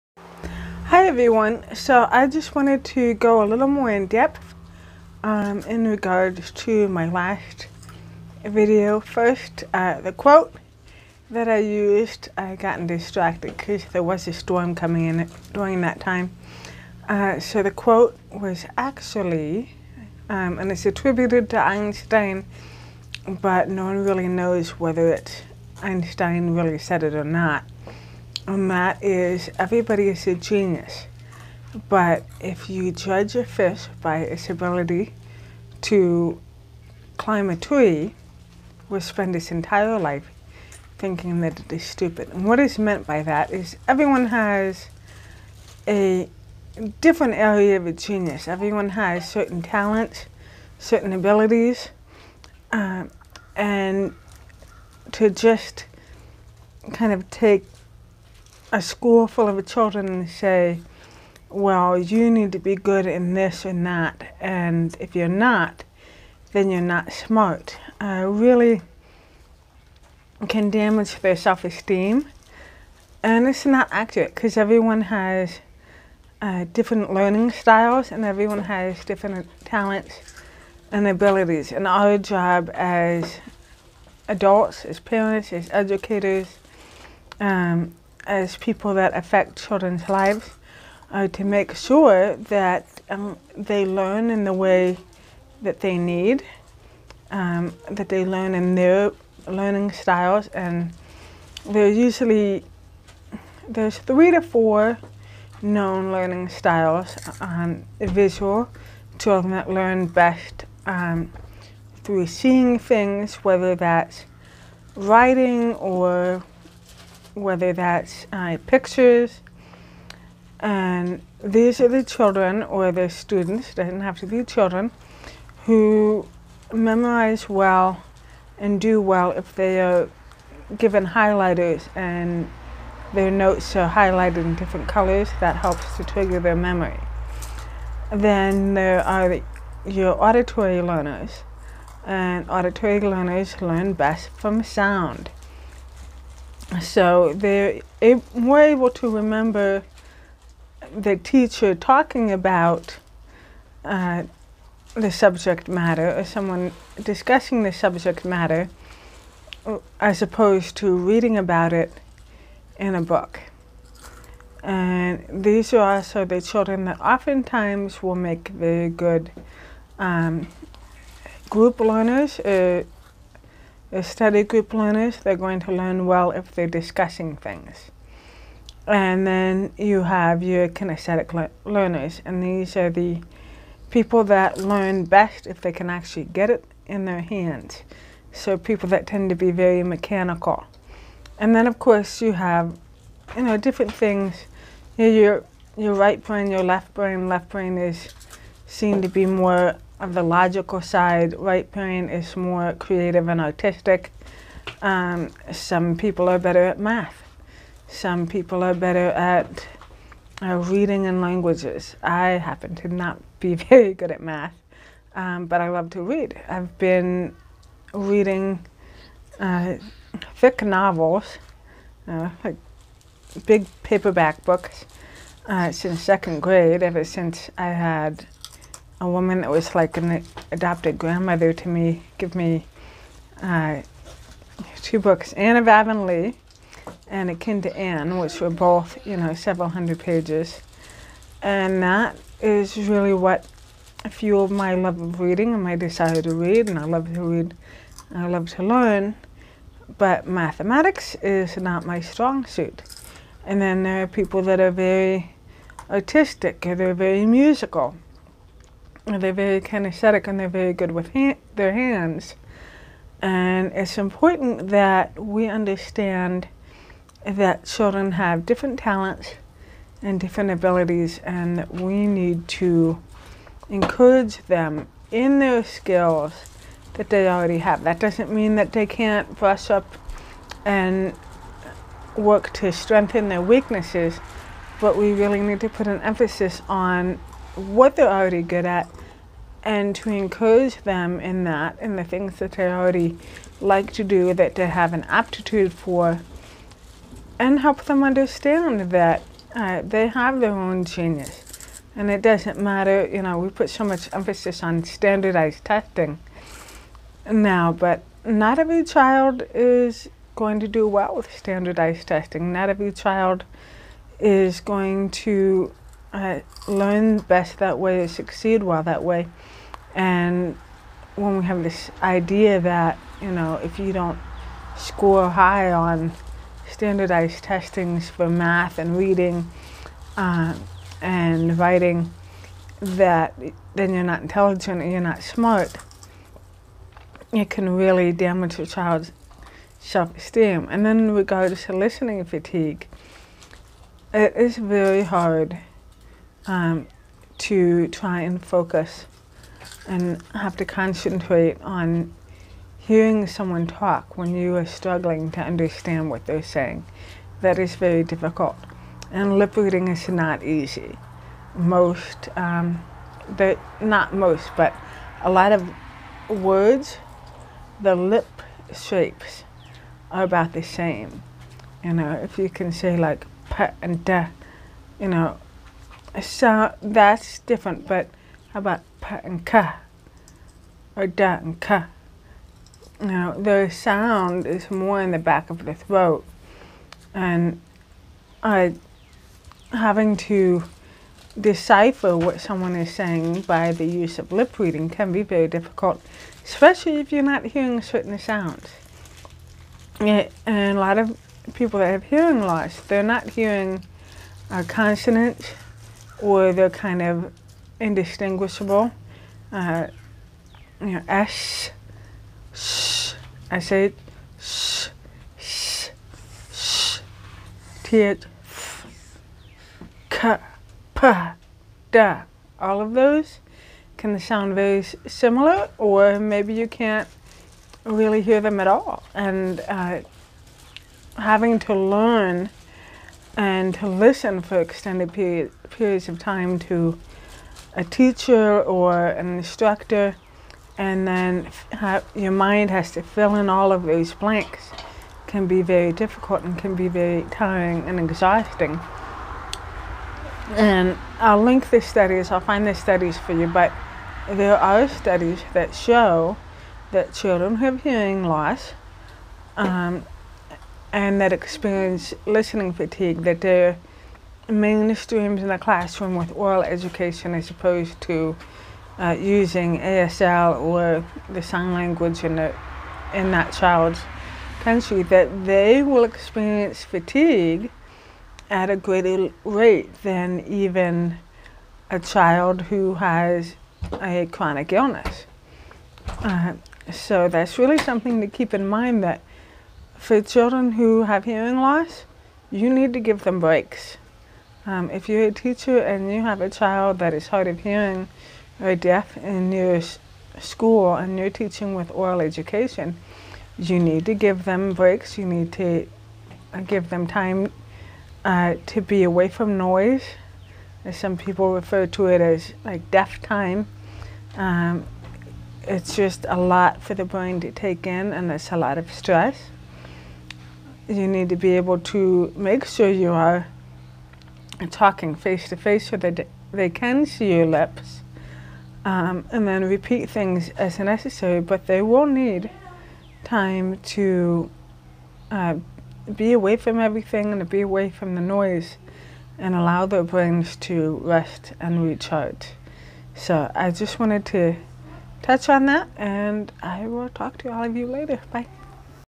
Learning styles and a child's zone of genius
'Every child is a genius.' We tend to judge children by standard measures of intelligence. But all children have different abilities and attributes. Some children are gifted in math or reading. Other children are musical. Or they may be athletic. It is important to understand what children are gifted in and what their learning styles are. When children have listening fatigue, they also have greater obstacles to overcome. We need to understand listening fatigue and how to help children with it.